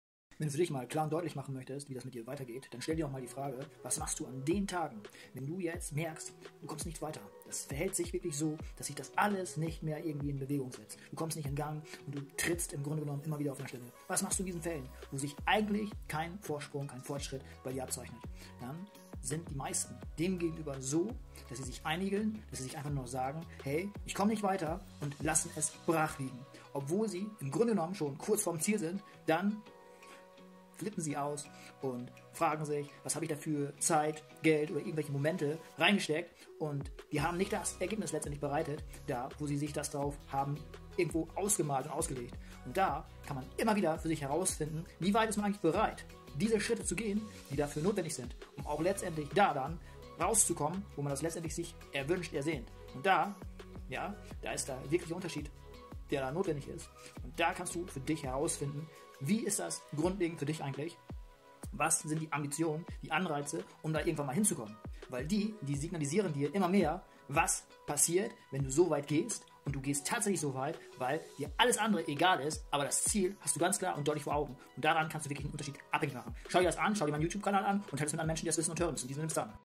Was machst Du, wenn Du nicht weiterkommst?
Was machst Du an den Tagen, wo Du nicht weiterkommst? Also, Du kommst nicht in Gang. 😃

Also wenn sich bei Dir kein Vorsprung oder Fortschritt abzeichnet? Finde die Ambitionen oder Anreize für Dich heraus, um da irgendwann mal hinzukommen! 🚀

Tiefer gehe ich für Dich auf dieses Thema im heutigen Video ein. Schau es Dir vollständig an!

Hast Du dazu irgendwelche Fragen? Schick mir einfach eine Nachricht oder besuche meine Seite! 👍